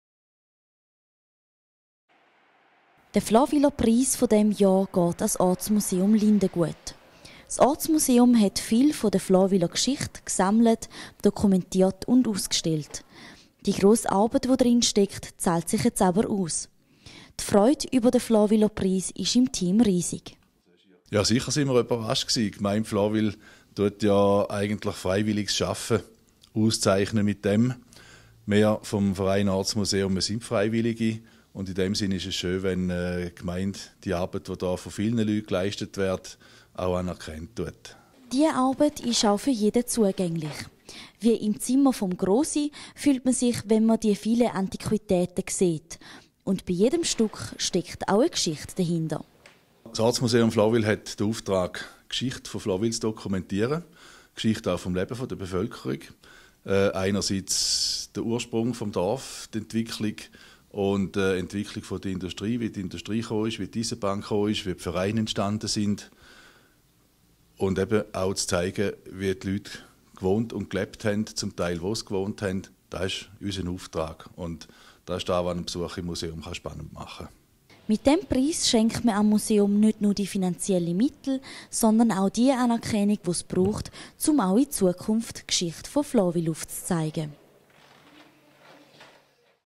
Flawiler-Preis
Das Ortsmuseum Lindengut wird mit dem "Flawiler-Preis" geehrt ...